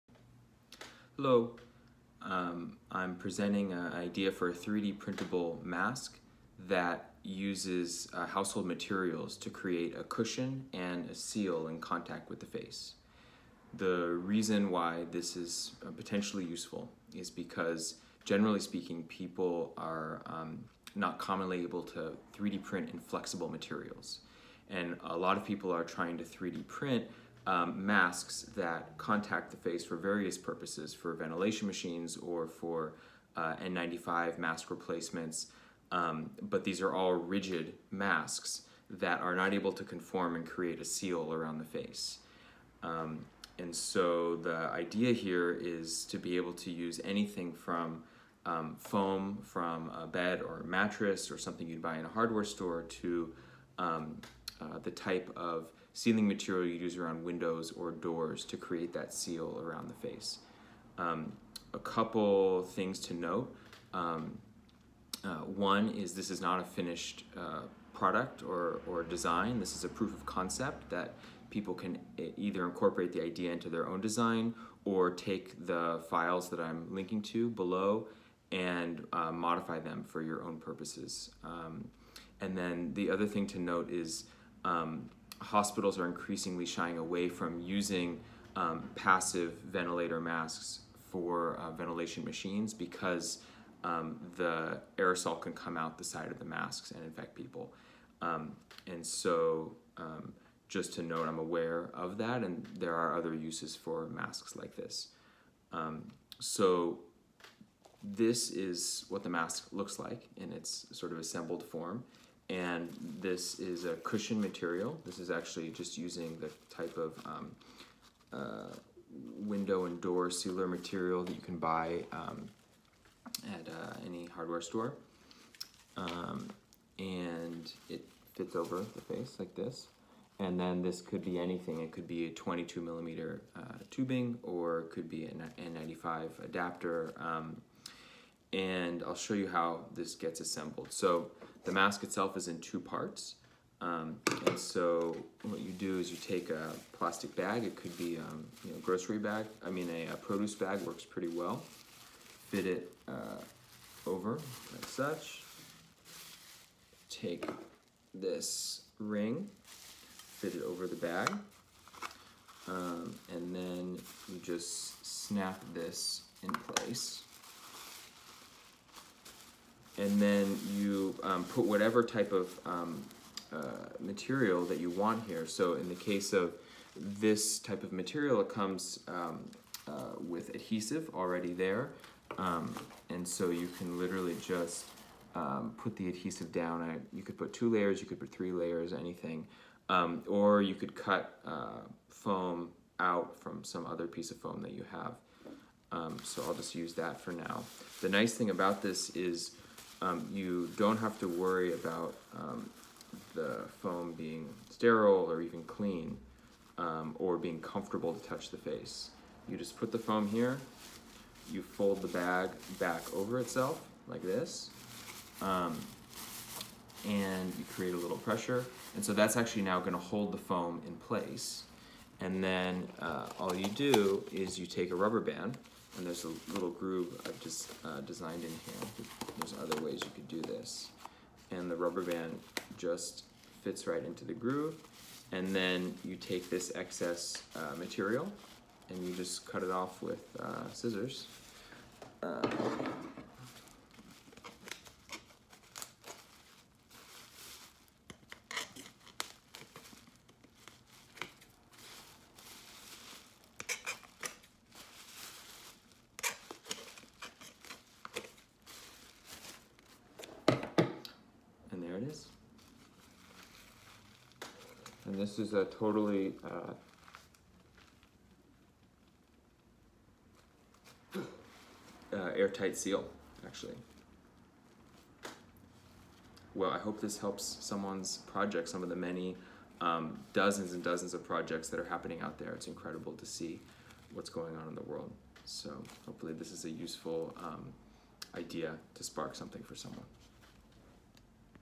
3d Printable Ventilator/N95 Mask With Flexible Seal From Household Materials
3D printable mask concept that uses readily available materials to create a cushion and seal where the mask meets the face. There are dozens of projects aimed at creating novel solutions to the problems created by the  COVID/Corona pandemic. Many of these projects use 3D printable masks, but those masks use rigid materials and are not able to conform to the face both for comfort and function. The idea presented here demonstrates how any type of foam, and a plastic bag, can be used to along with a 3D printed mask design to create a comfortable fitting mask.

This is not a complete design, its just a proof of concept. Please feel free to run with this idea, and incorporate it into your designs as useful. It's currently designed with a 22mm tube fitting, but could be repurposed for an N95 respirator or any other project needing a mask.

I doubt this would be needed in a hospital setting. They are not using masks for mechanical ventilation systems as pressured are can be forced out of the side of the mask increasing risk of exposure. This is probably more in the DIY camp of project focused on last resort at home care or 3rd world countries.

Designed in collaboration with some amazing folks based out of the Santa Barbara Center for Arts Science and Technology in Santa Barbara